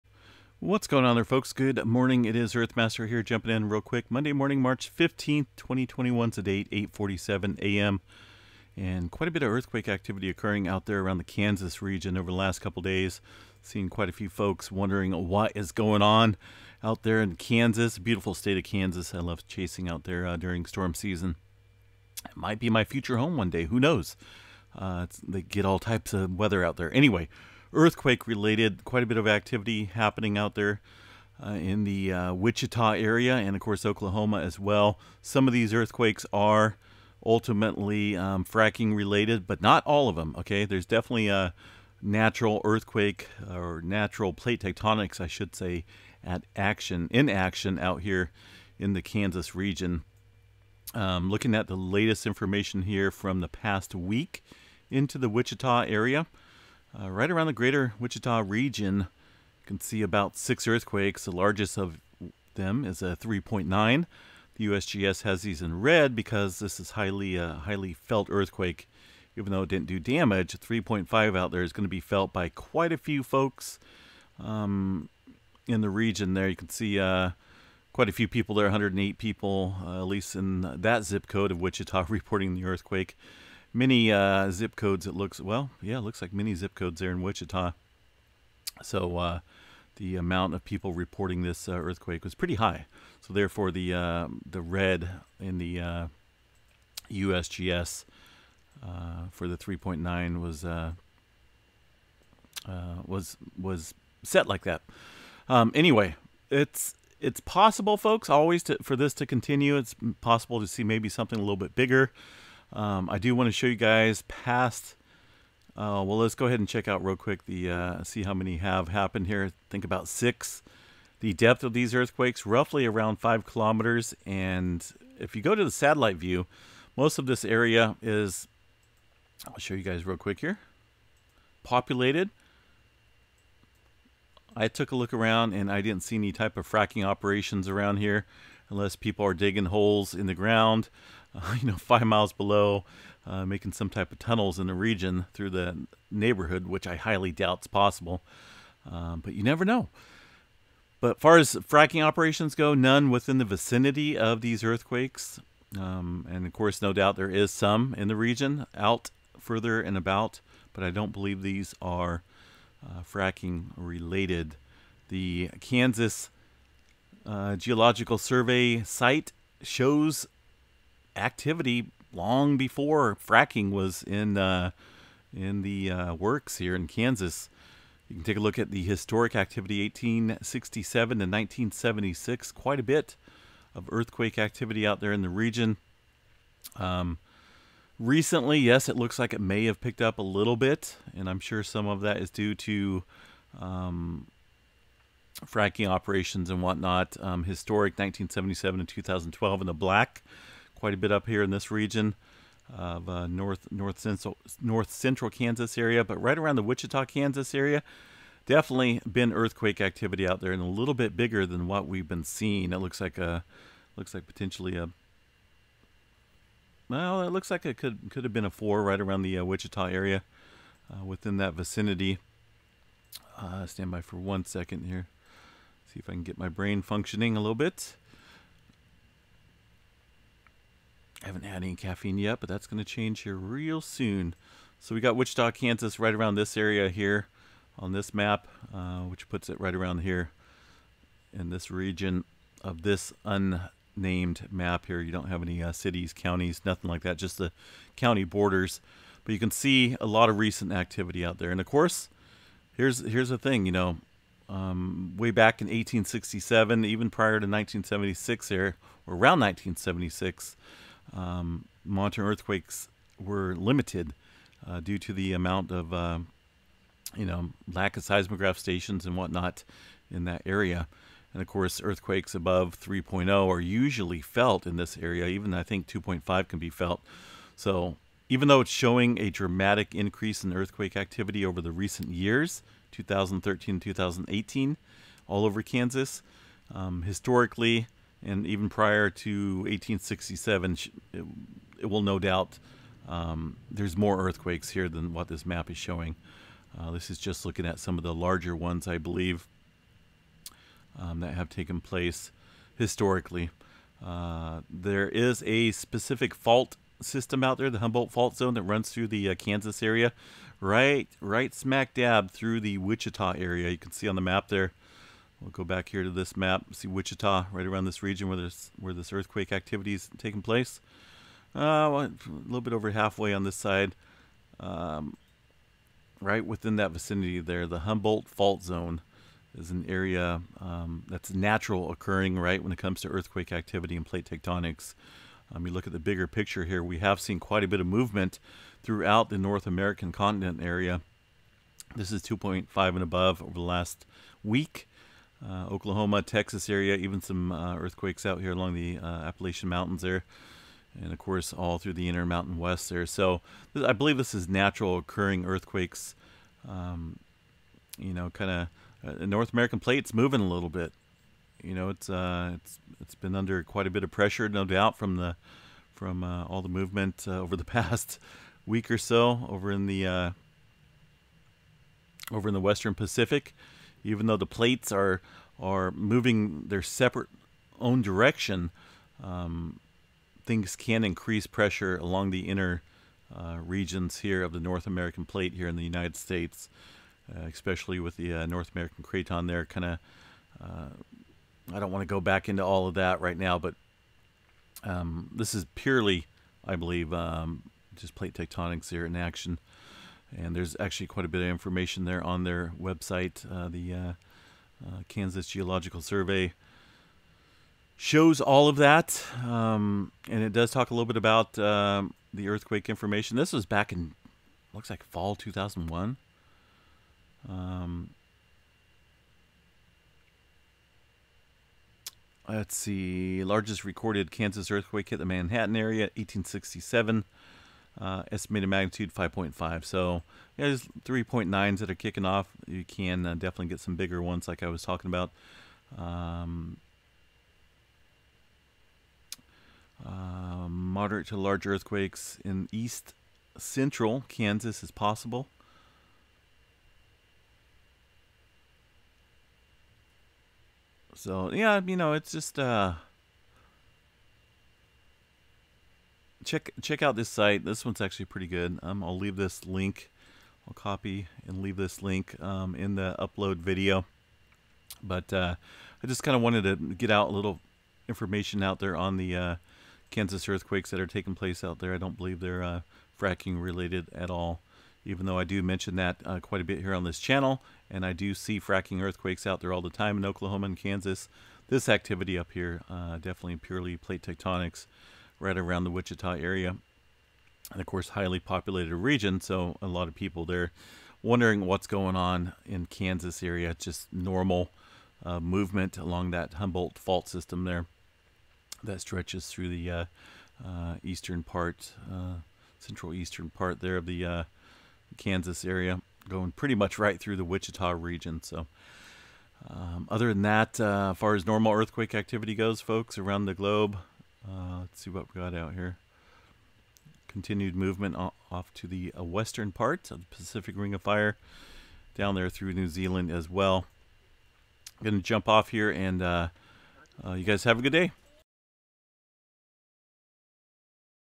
Kansas Earthquakes.. What's going on? 3/15/2021
The largest recorded Kansas earthquake hit the Manhattan area in 1867, toppling chimneys, cracking walls and foundations, and inflicting several minor injuries. The estimated magnitude 5.5 earthquake was felt as far away as Dubuque, Iowa. Since 1867, more than 25 earthquakes with origins in Kansas have been felt; before then, earthquakes generally went unreported.
While most earthquakes that occur in Kansas are mild, a moderate to large earthquake in east-central Kansas is possible. And with critical structures such as major dams and power plants, even a low-probability earthquake has to be taken seriously.